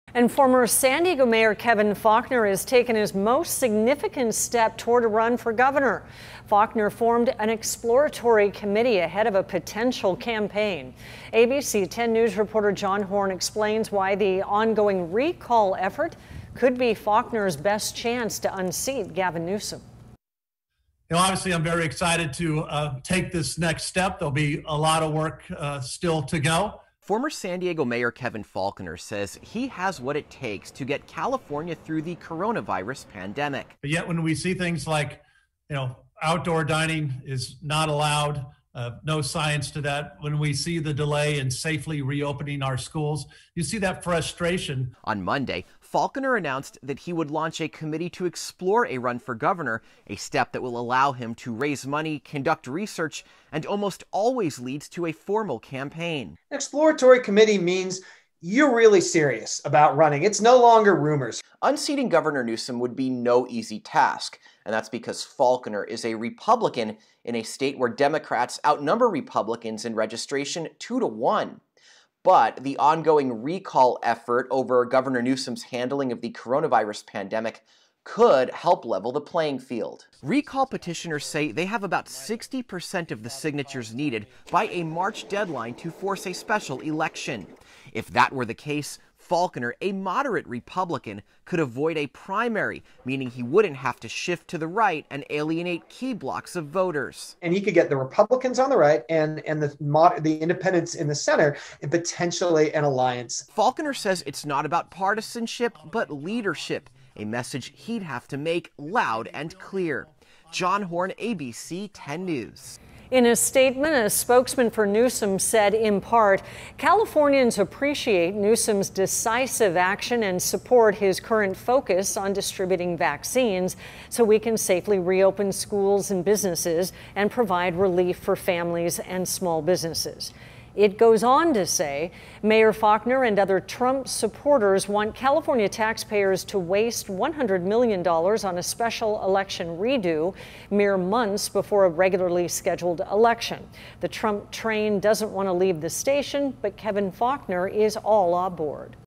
Faulconer takes key step toward gubernatorial run
Former San Diego Mayor Kevin Faulconer has taken his most significant step to date as he weighs whether to run for governor of California